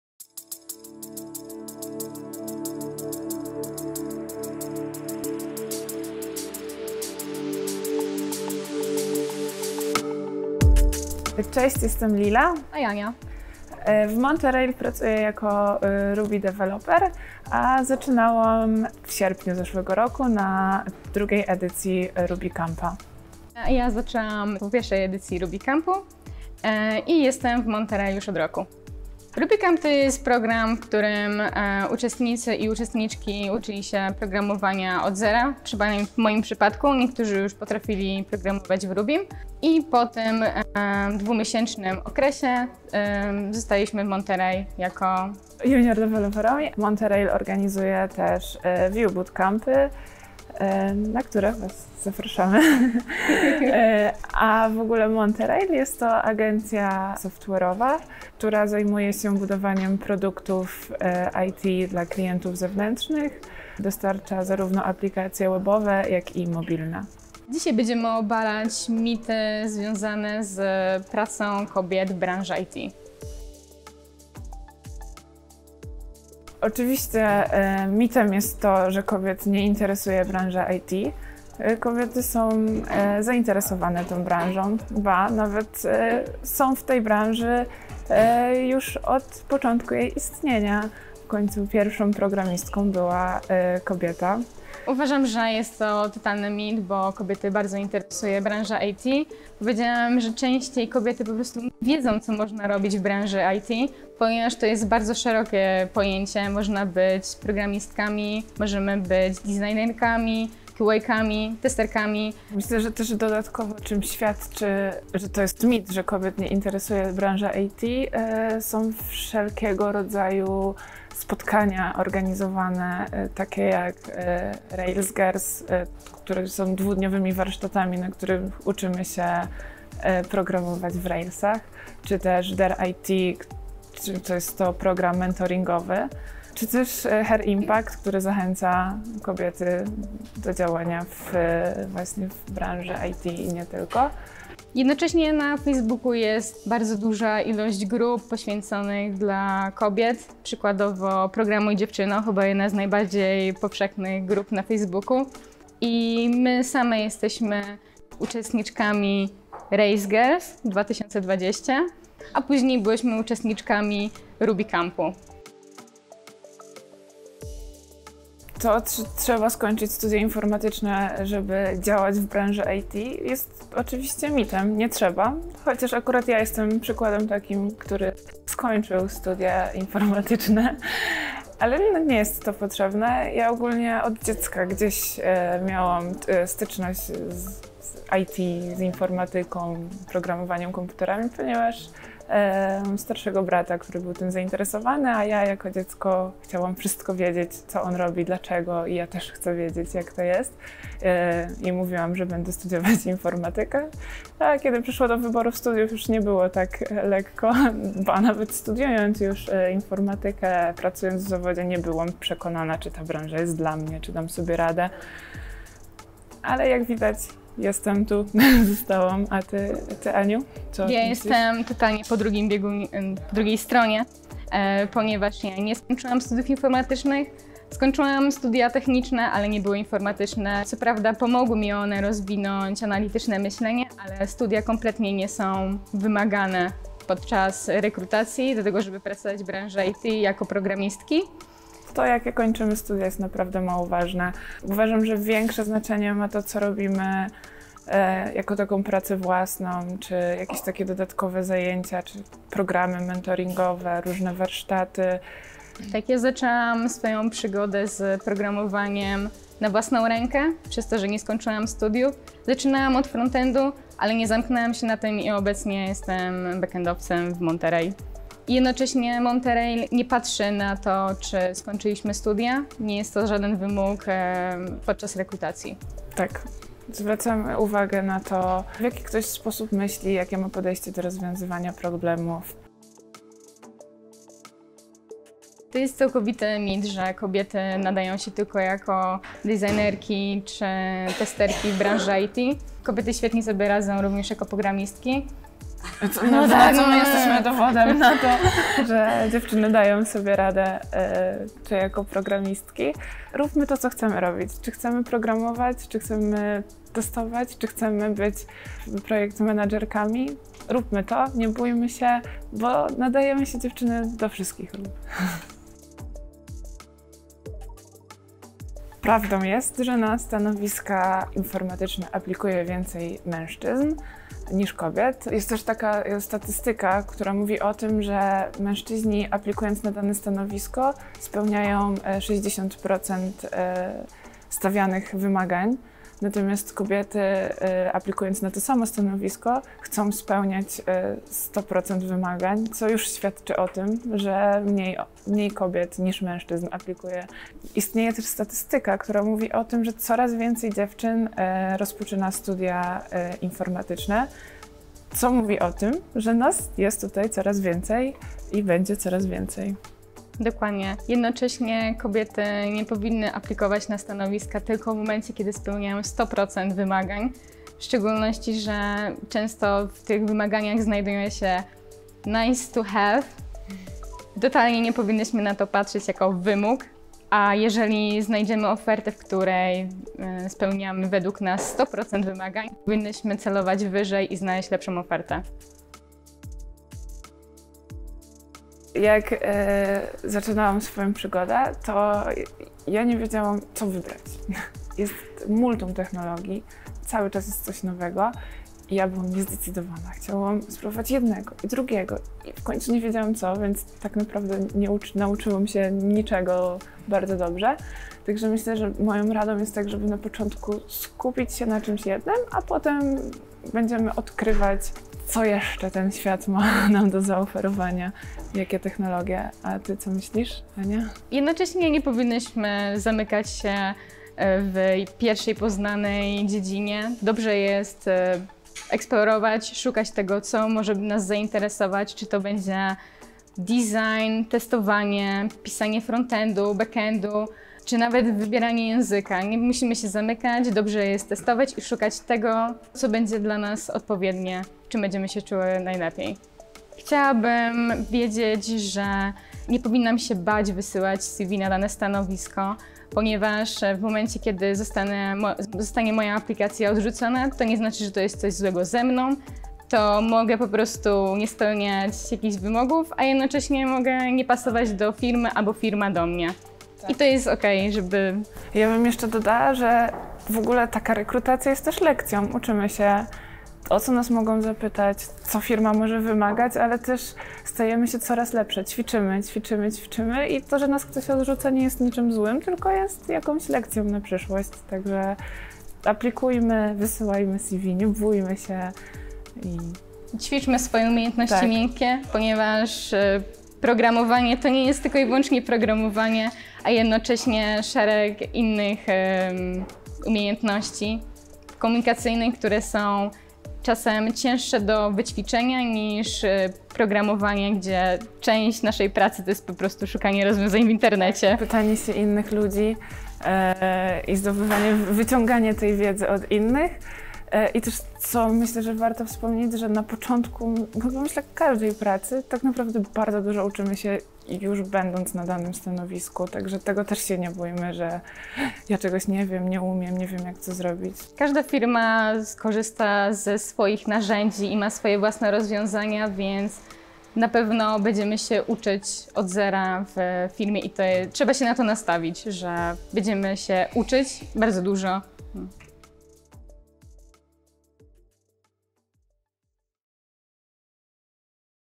Debunking the myths about women in IT - Ania Kotowicz & Lila Łomnicka | Her Impact
“Women are not into IT”
“You need to have IT degree to get your first job”
“Women are best suited for designer or tester roles”

And so on. You’ve probably heard all these things when starting a career in IT or when working within this industry already. It’s time to dispel these myths! How about we just listen to what women say about it? Together with Her Impact, the first platform supporting women's development, we did it and heard some inspiring stories.

Anna Kotowicz and Lilla Łomnicka, Monterail Ruby on Rails developers commented on popular belief about women in IT, shared their development path and advice for beginners. They both joined Monterail as juniors after successfully completing the Ruby Bootcamp – the intensive 2-month course dedicated to getting the necessary skills and knowledge about software development.

Does it sound like your possible first step into IT? We’re recruiting now for the September edition, find out the details